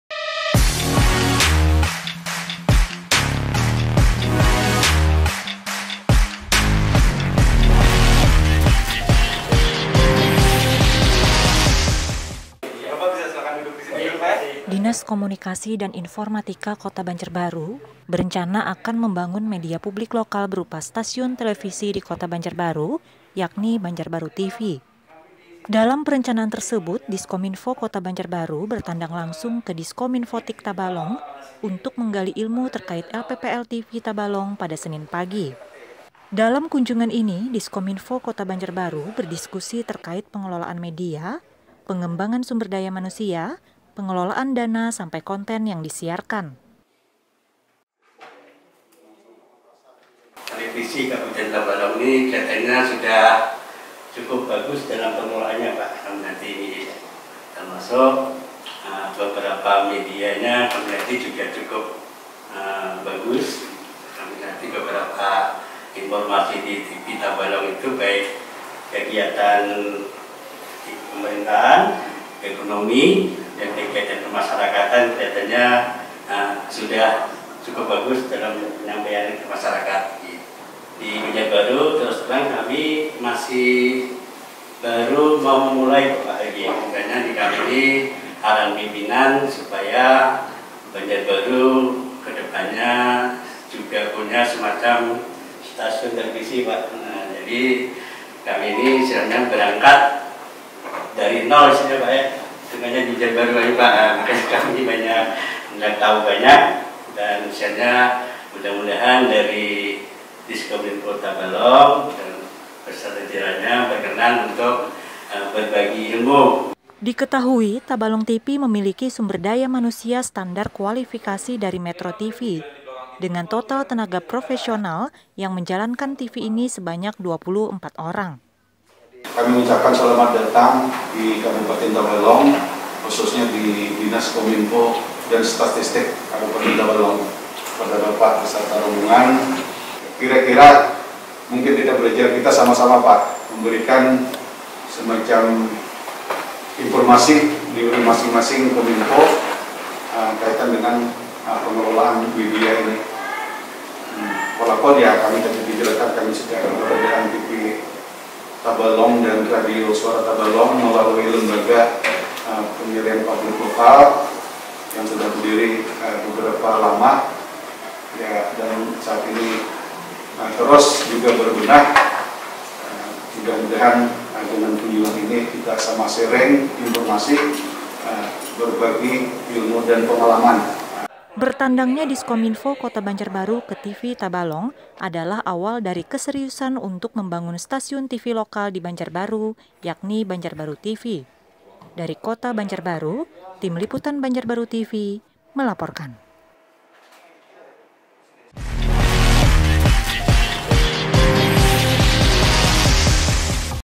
Diskominfo Kota Banjarbaru Bertandang Ke TV Tabalong
Dinas Komunikasi dan Informatika Kota Banjarbaru, berencana akan membangun media publik lokal berupa stasiun televisi di Kota Banjarbaru, yakni Banjarbaru TV.

Dalam  perencanaan tersebut, Diskominfo Kota Banjarbaru bertandang langsung ke Diskominfotik Tabalong, untuk menggali ilmu terkait LPPL TV Tabalong pada Senin pagi.